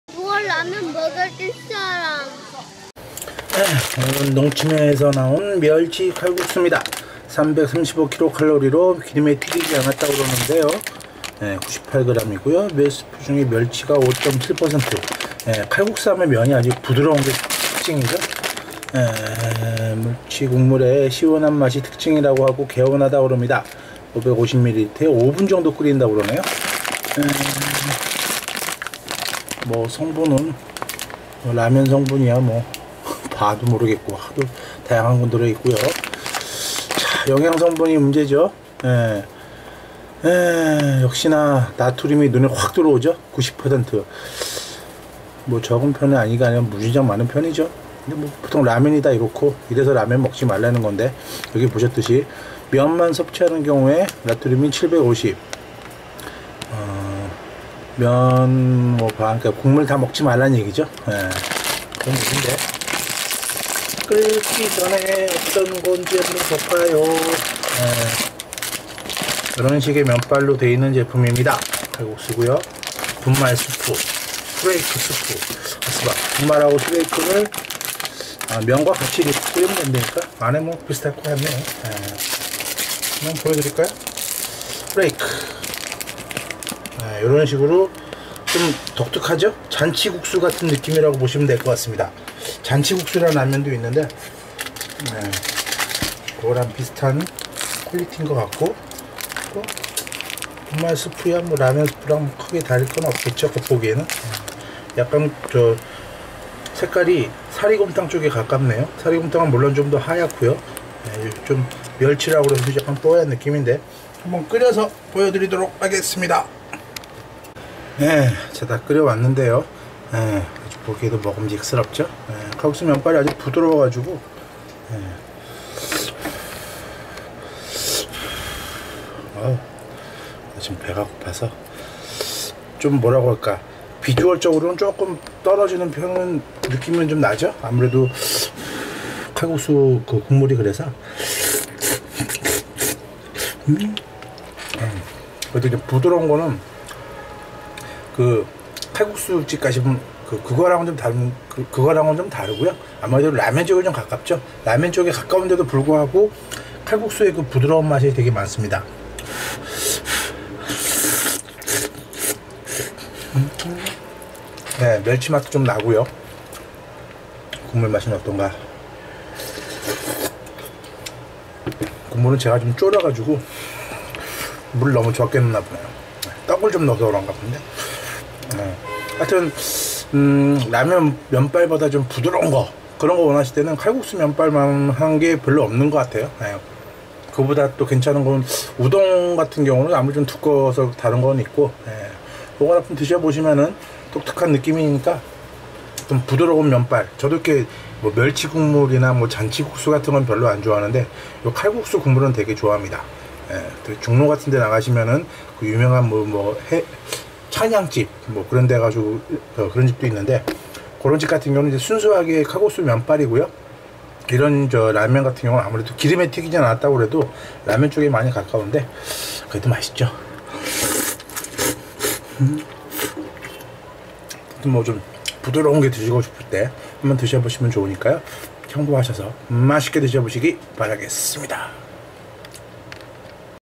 농심 멸치 칼국수, 기름에 튀기지 않은 부드러운 면발의 라면 시식기
농심에서 나온 칼국수인데, 멸치 국물로 시원하고 면발이 상당히 부드러운 제품으로 자주 먹으면 질리기는 하지만 가끔 드셔보시면 부드러운 면발이 상당히 마음에 드는 제품입니다.
이 제품외에도 다른 칼국수 제품도 있는데, 종종 별미로 드셔보시면 좋은데, 물론 나트륨이 많기는 하니 이점은 참고하시기 바라겠습니다.

인터넷 쇼핑몰에서는 꽤 저렴한 가격에 판매를 하는데, 박스로 사다놓고 종종 먹어도 좋을듯...^^